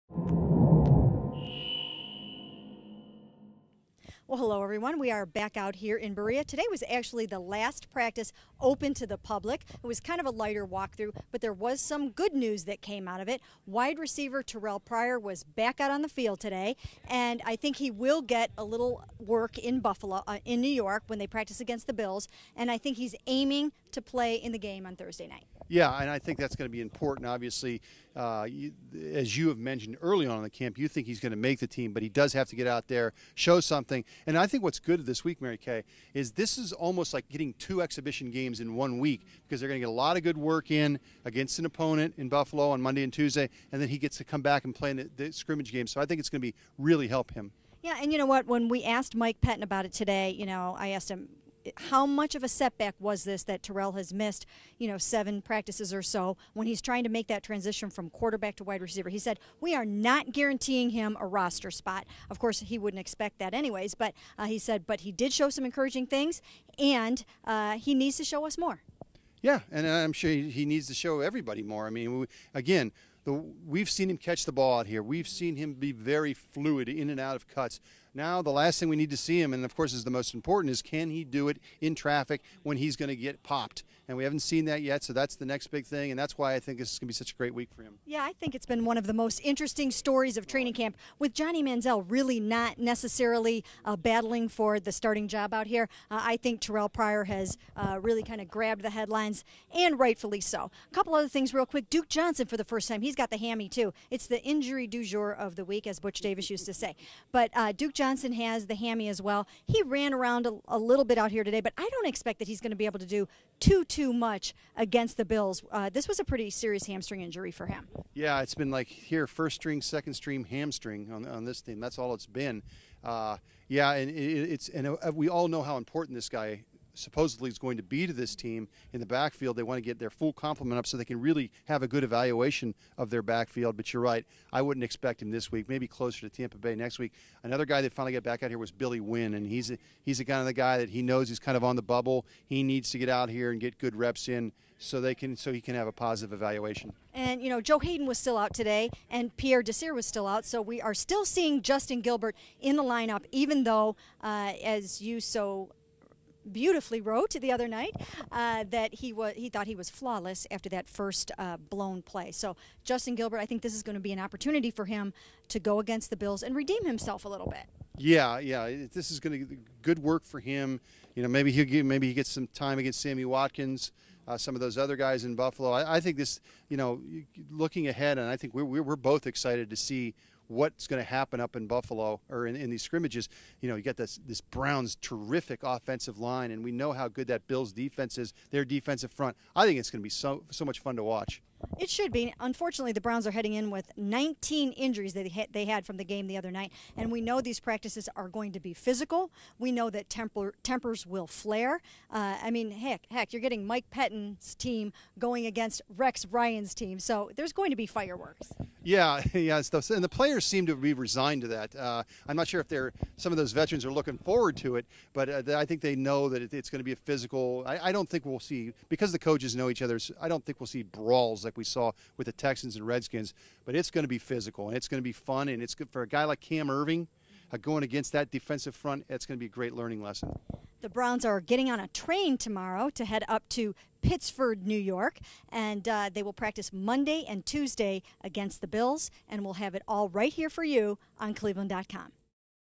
Mary Kay Cabot and Tom Reed on Day 12 of Browns training camp
Cleveland.com&apos;s Mary Kay Cabot and Tom Reed break down everything that happened on Saturday at Cleveland Browns training camp.